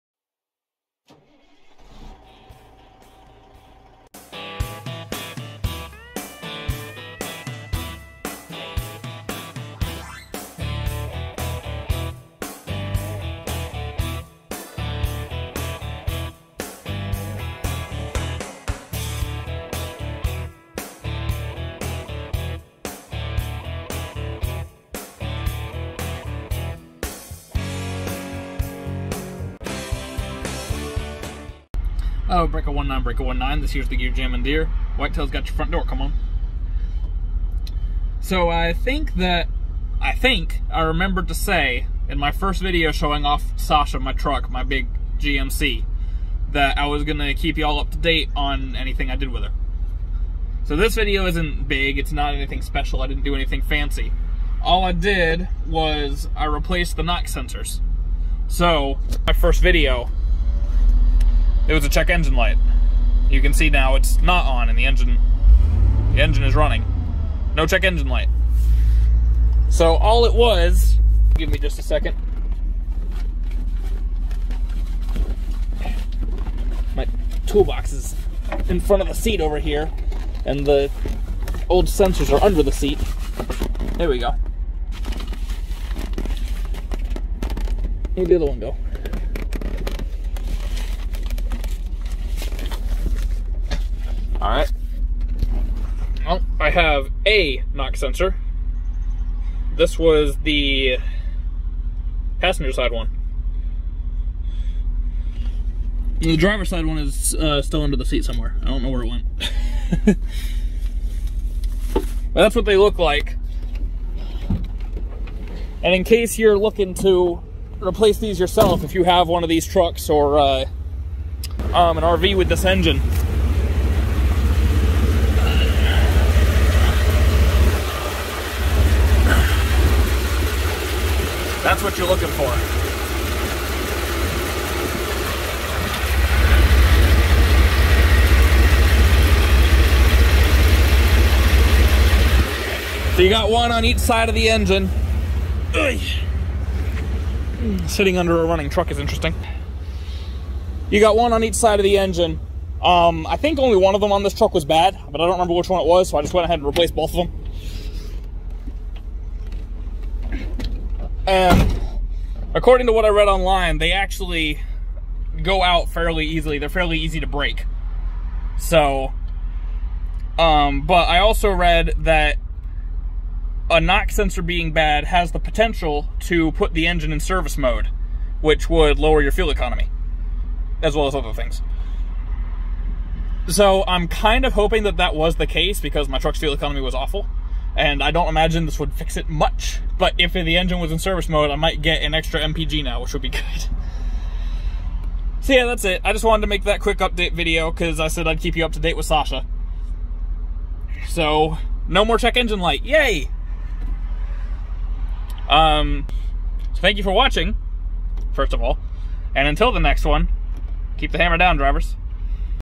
No More Check Engine Light! | Sasha Progress
Sasha's check engine light has been on ever since the first time I drove 'er.
Anyway, I thought it was high time I did something about it.